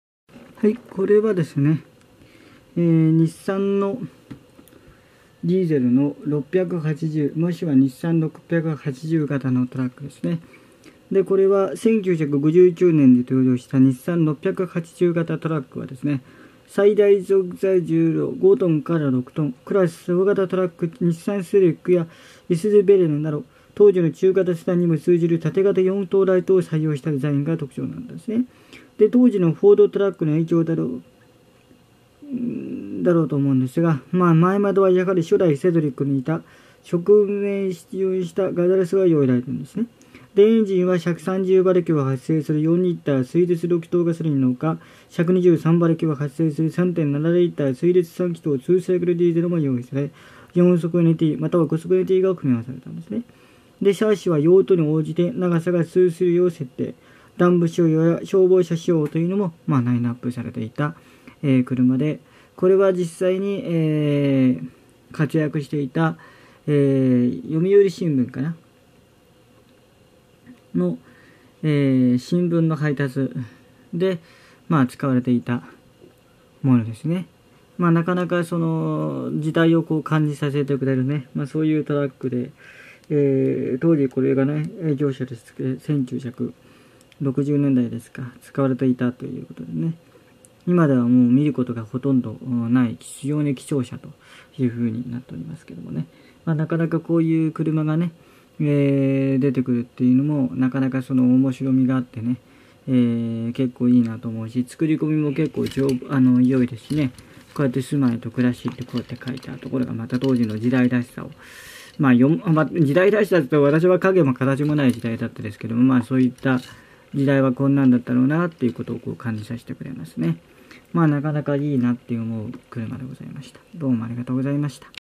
発達知的障害が感じた昭ＴＬ－Ｖトミカ「第891弾」日産６８０型トラック読売新聞
車種解説、５ｔ～６トンクラスのトラックで、日産セドリック、いすゞべレルを参考に縦型4灯ライトを採用したデザインで、水冷3気筒エンジン2サイクルディーゼルエンジンを搭載し、130馬力～120馬力を搭載し4速～５速マニュアルを搭載し、フォードトラックの影響を受けたトラックです。